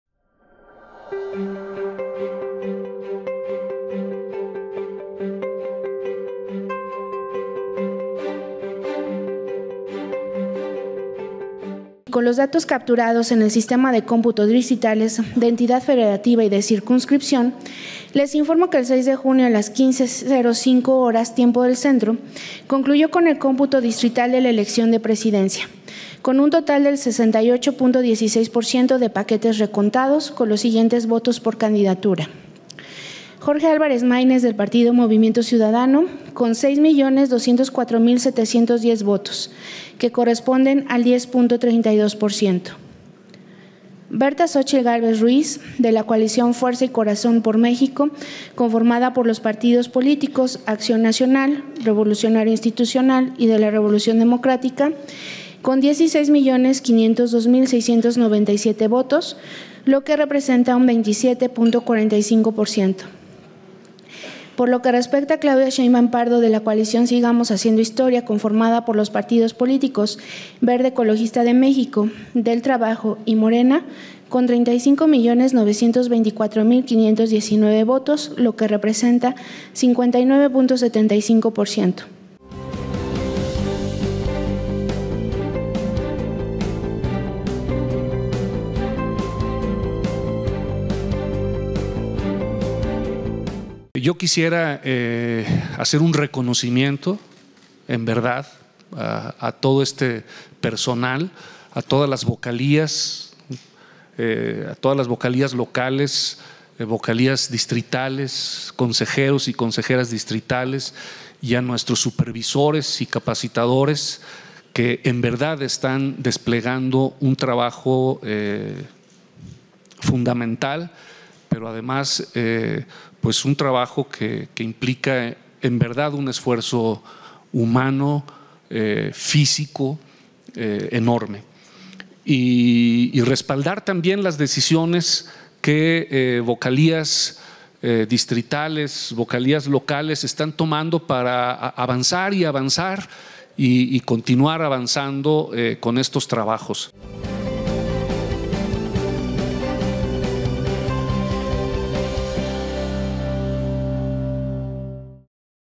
El INE da a conocer el resultado de los cómputos distritales de la elección a la Presidencia
El INE da a conocer el resultado final de los cómputos distritales de la elección a la Presidencia de la República. El resultado es congruente con los del PREP y las estimaciones del conteo rápido.